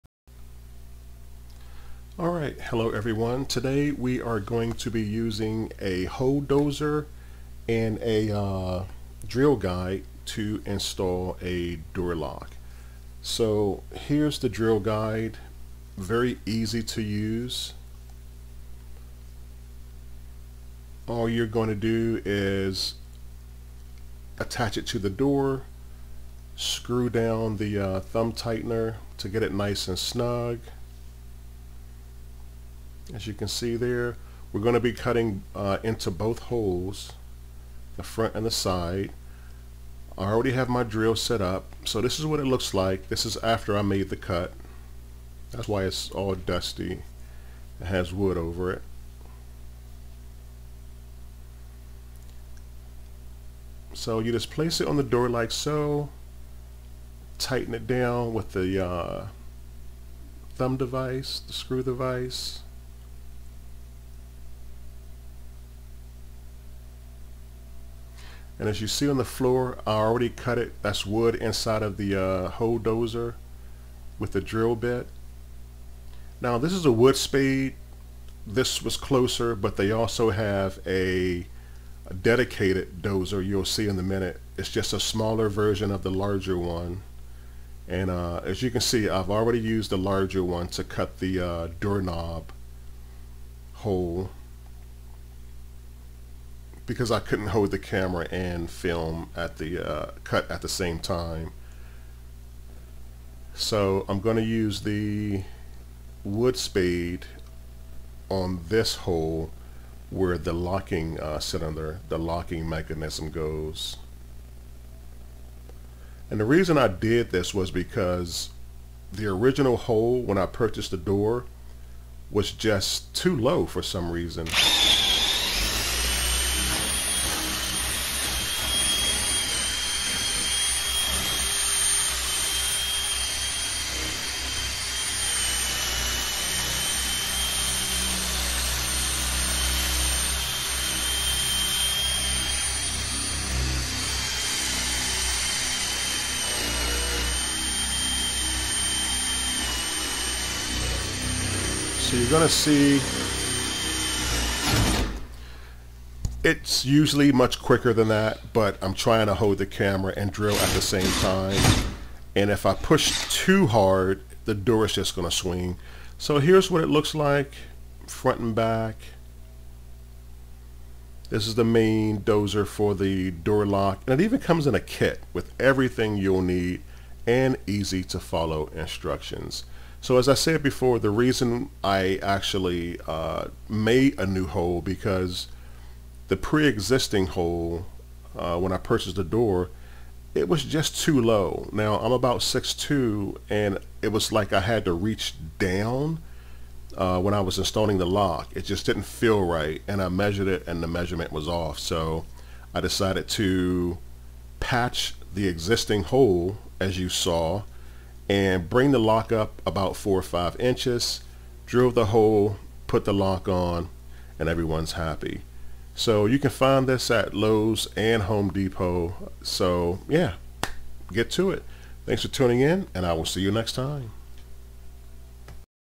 DIY Using A Hole Dozer For Lock Installation
In this video we discuss using a Hole Dozer for a lock install. The existing Prehung doors hole sat extremely low on the door, the hole was patched using compound then sanded and painted over.

Be sure to check you States Building? Residential Code on Doorknob Heights, yes there is a such a thing.

"The International Building Code has established doorknob height between 34 and 48 inches above a finished floor. Installers typically drill the hole centered 36 inches above the floor -- if not specifically instructed to do so differently. Exceptions to the rule include doors that provide egress into swimming pools or other water features. In this instance, the doorknob is installed 48 to 54 inches above the floor, specifically to raise the knob out of the reach of small children."

Excerpt from Hunker Article.👆🏽